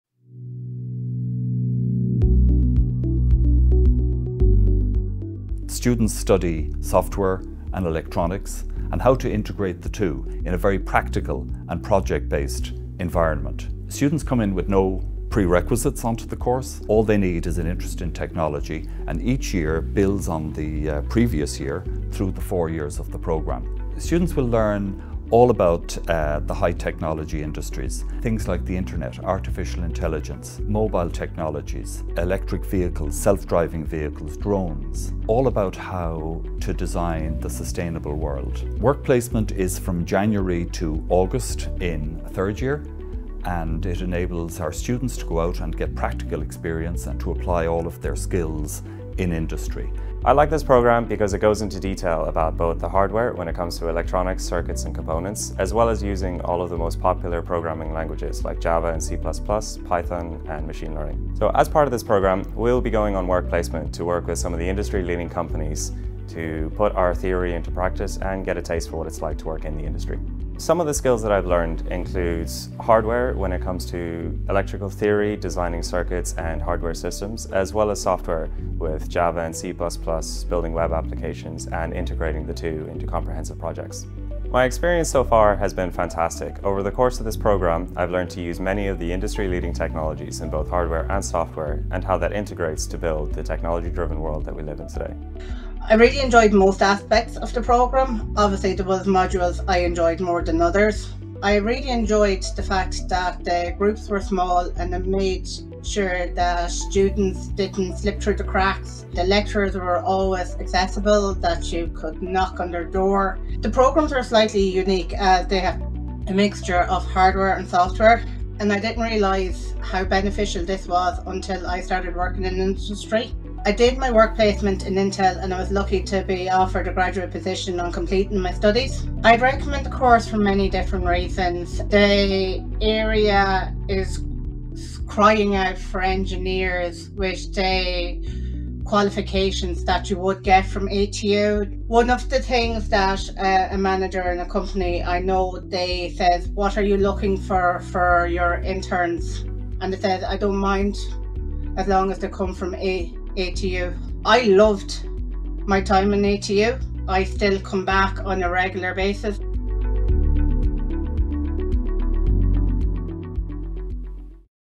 AU642 & AU542 BEng (Hons) and BEng Software and Electronic Engineering.
This four year honours degree prepares graduates to work as software and electronic engineers in a variety of research and development settings within the tech industry. ATU has one of the most highly valued Software & Electronic Engineering qualifications in Ireland. You will learn about software, computers, electronics, the Internet, and the cloud, from the basic transistor to the Internet of Things. There is a particular focus on state-of-the-art technologies and engineering techniques.

Your personal and professional development form key components of the programme. We pride ourselves on our innovative learning environment which uses team and problem based techniques; this means less teaching and more learning for you. An eight-month work placement is offered in the third year of the programme. This enables students to apply their knowledge and develop their skills within high tech work environments such as Cisco, Intel, Ericsson, Analog Devices, SAP, Avaya, Valeo and Boston Scientific. Alternatively, students can avail of an Erasmus+ study placement in a partner college in Europe.